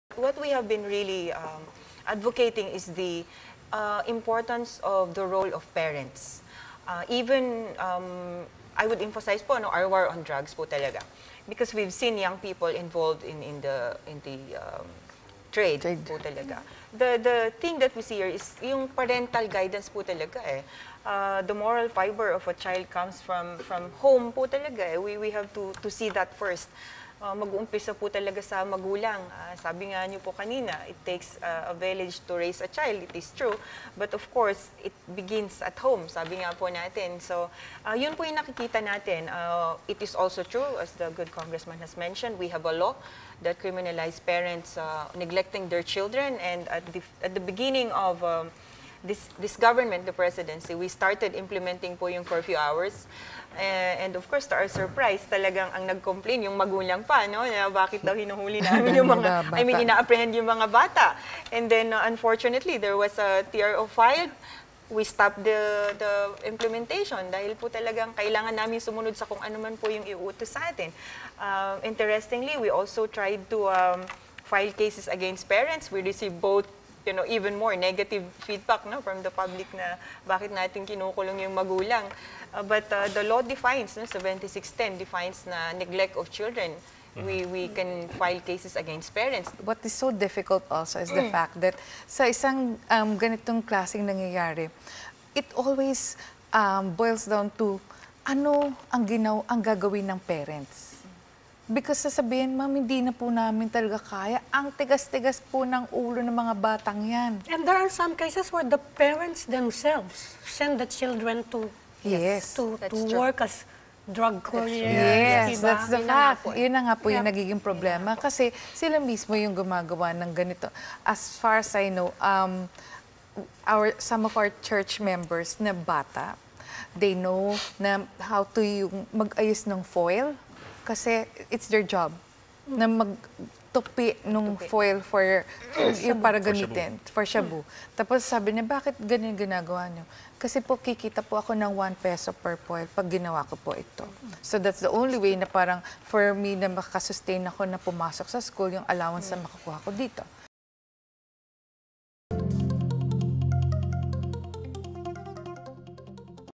What is the role of parents in all this?
PNP Deputy Spokesperson PSupt. Kimberly Molitas and Child Psychologist Dr. Camille Garcia stress the role of parents in addressing juvenile delinquency.